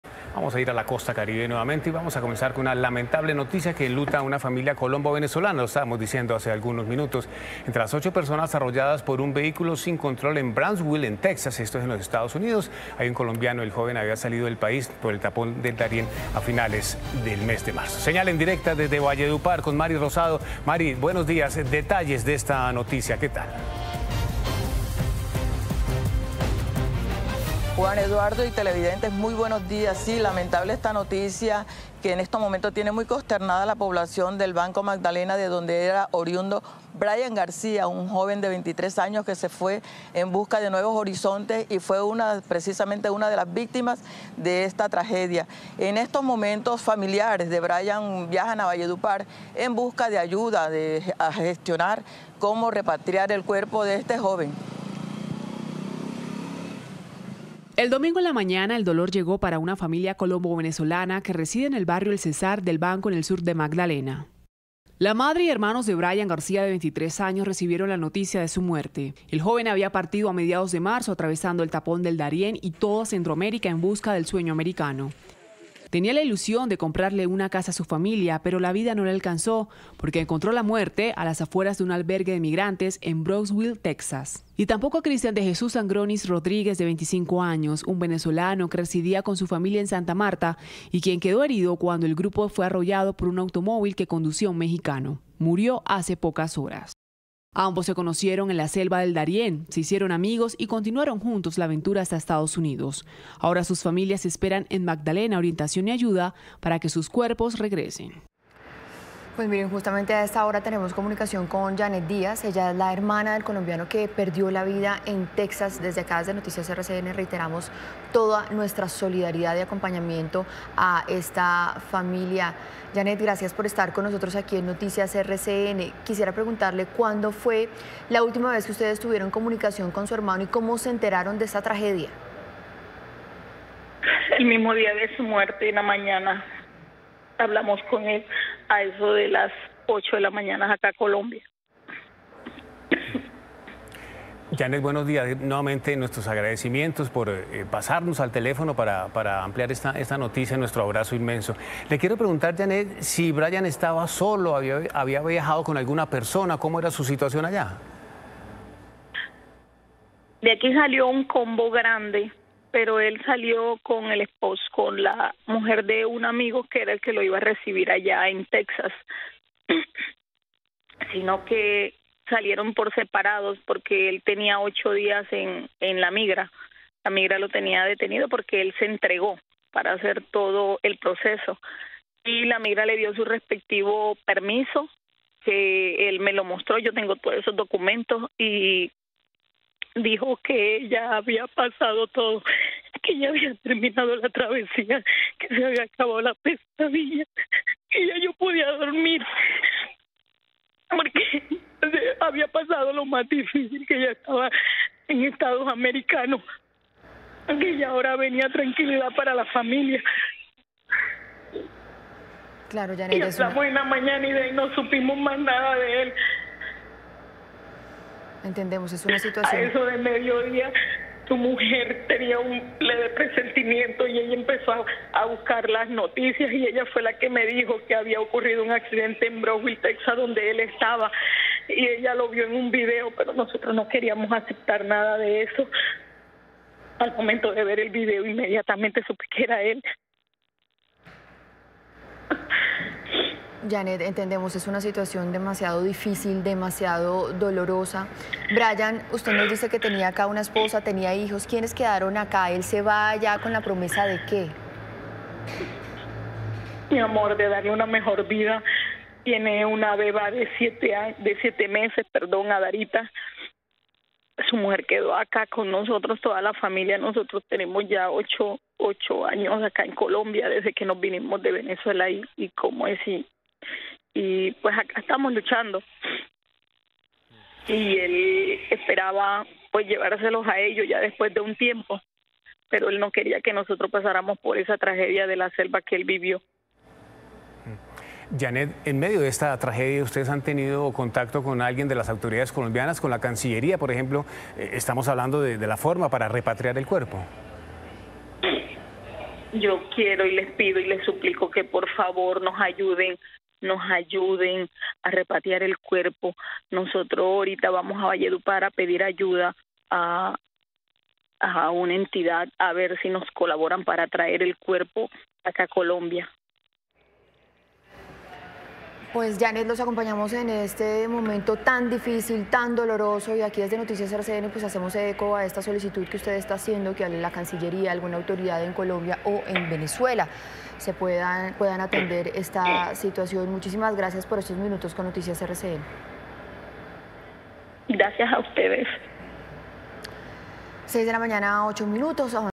Joven colombo-venezolano murió tras ser arrollado a las afueras de un albergue en Texas
NoticiasRCN | El joven colombiano identificado como Brayan García había salido del país para trasladarse a los Estados Unidos a finales de marzo.